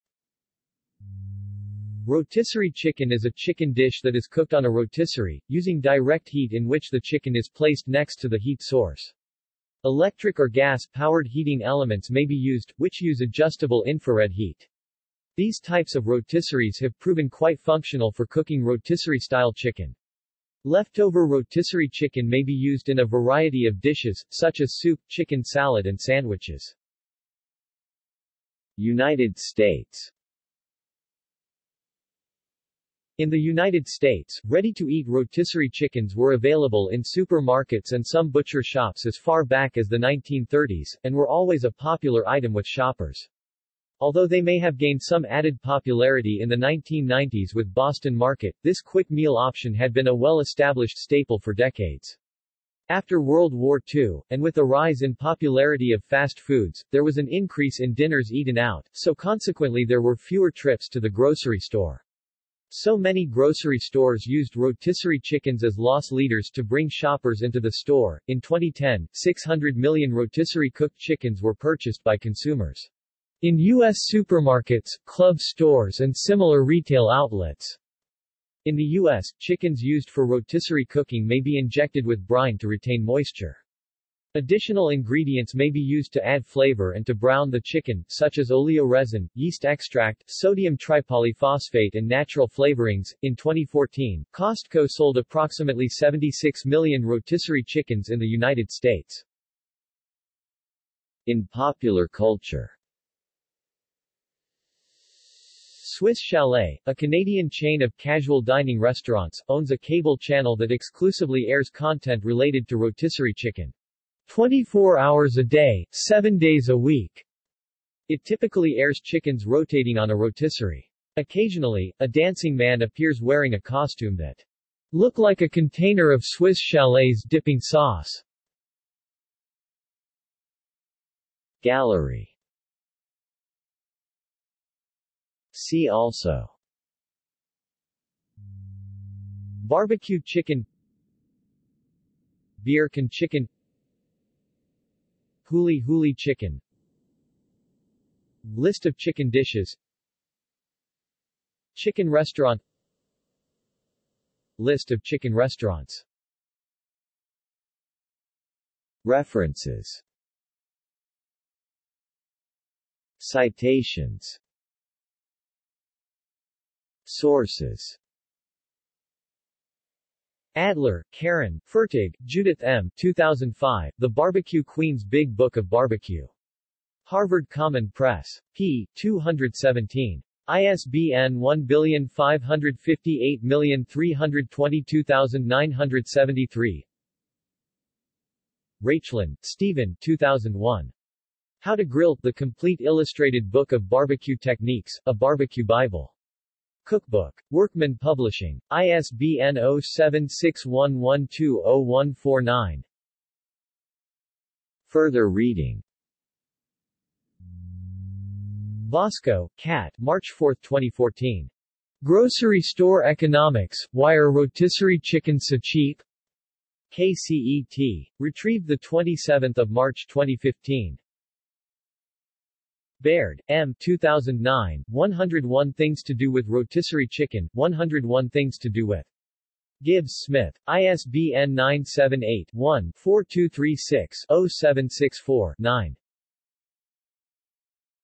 Rotisserie chicken | Wikipedia audio article
This is an audio version of the Wikipedia Article:
Rotisserie chicken

SUMMARY
Rotisserie chicken is a chicken dish that is cooked on a rotisserie, using direct heat in which the chicken is placed next to the heat source. Electric- or gas-powered heating elements may be used, which use adjustable infrared heat. These types of rotisseries have proven quite functional for cooking rotisserie-style chicken. Leftover rotisserie chicken may be used in a variety of dishes, such as soup, chicken salad and sandwiches.